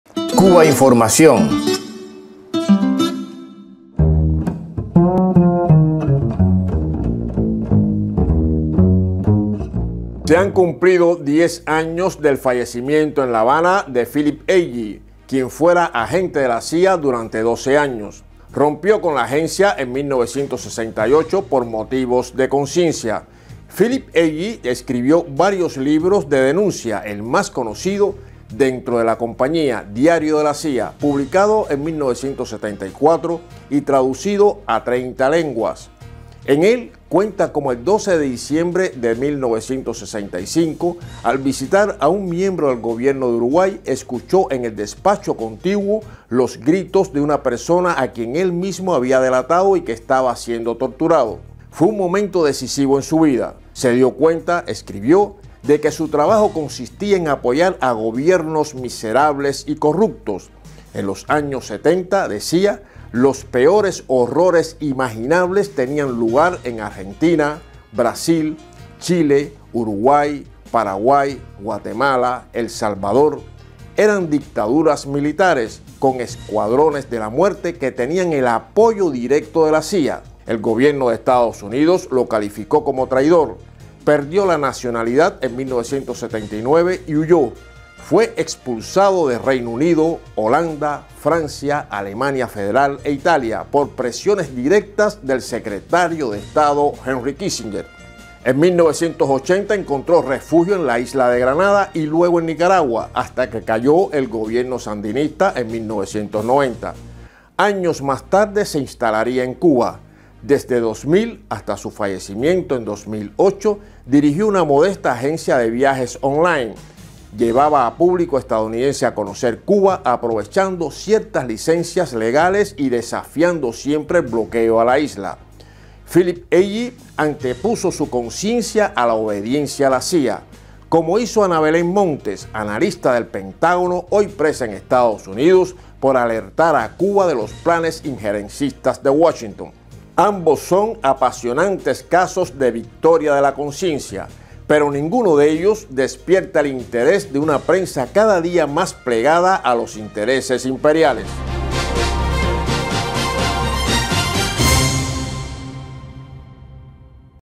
Philip Agee y Ana Belén Montes: la victoria de la conciencia, invisible para la prensa
Cubainformación TV – Basado en un texto de Pedro Martínez Pírez – Blog “Descubriendo verdades”.- Se han cumplido diez años del fallecimiento en La Habana de Philip Agee, quien fuera agente de la CIA durante 12 años. Rompió con la Agencia en 1968 por motivos de conciencia. Edición: Ana Gil. Fuente